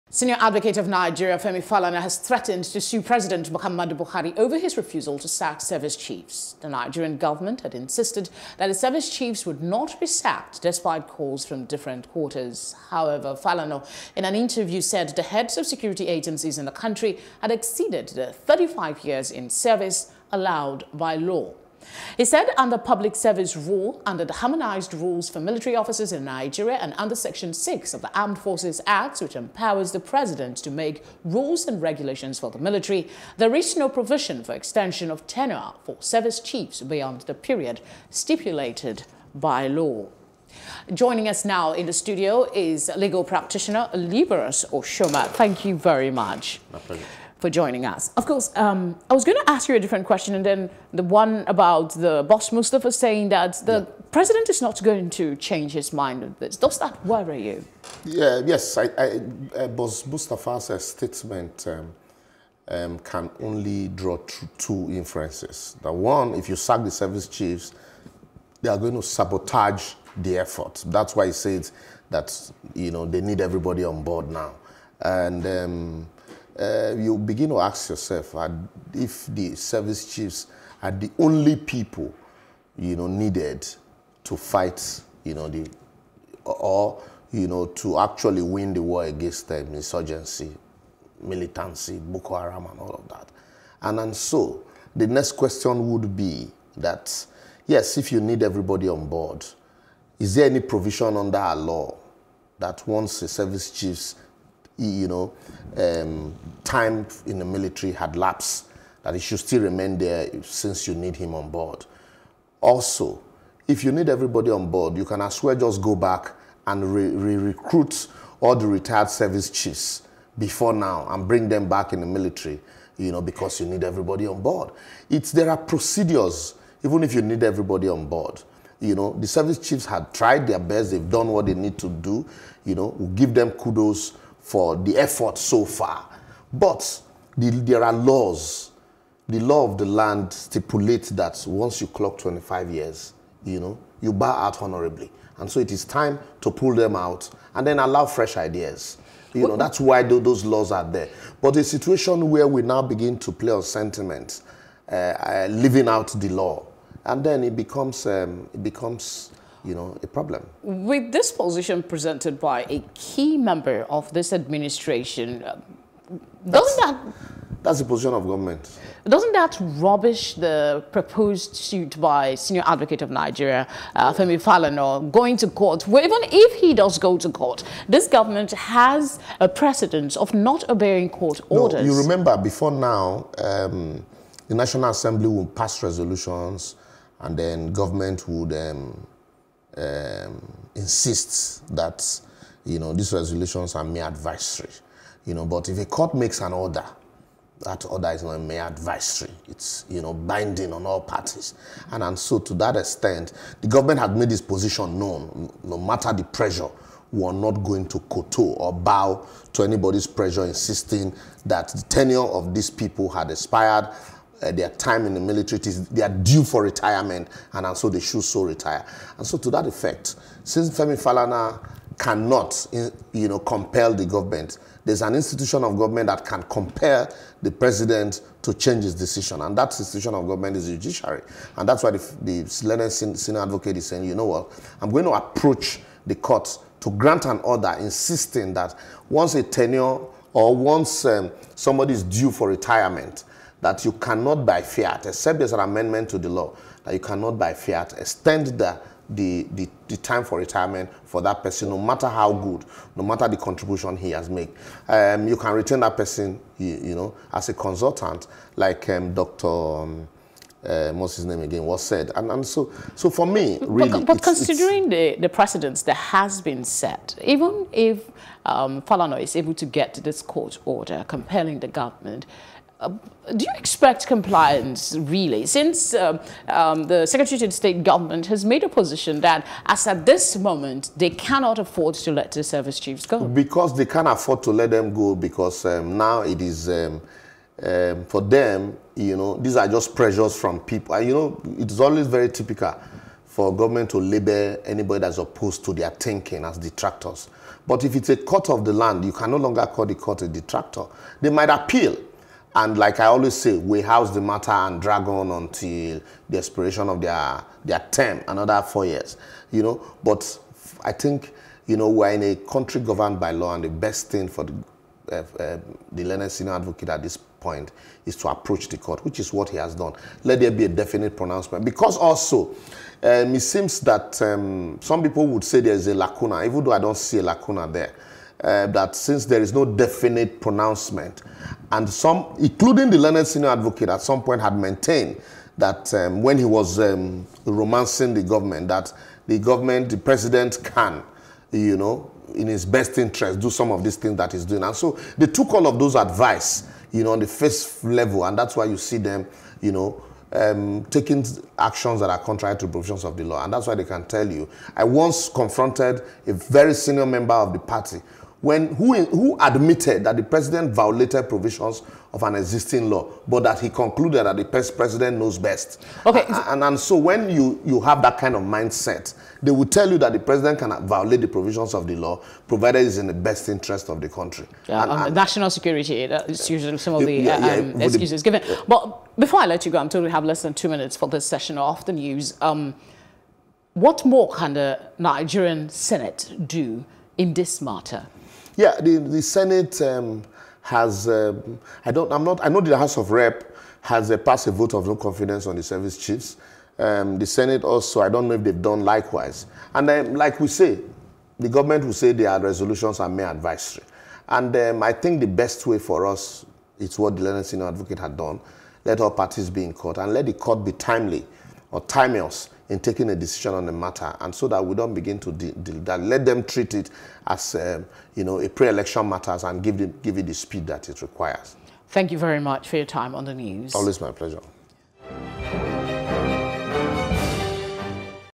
Femi Falana To Sue Buhari For Refusing To Sack Service Chief
Senior Advocate of Nigeria, Femi Falana, has threatened to sue President Muhammadu Buhari over his refusal to sack service chiefs.

The Nigerian government had insisted that the service chiefs would not be sacked despite calls from different quarters.

Liborous Oshoma, legal practitioner joined us in the studio.